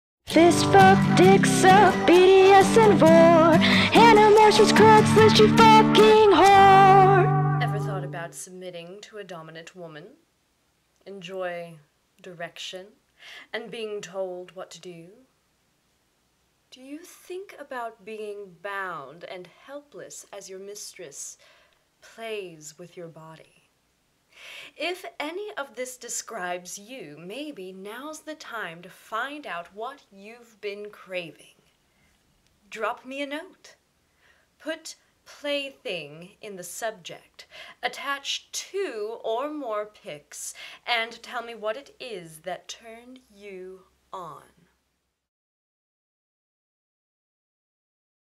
Hannah Morris Reads Personals: Oklahoma Domme
Hannah Morris Reads Craigslist is my way of practicing my acting skills (cold reading, accents, etc). I do NOT own this material.

she's a domme and ready for you.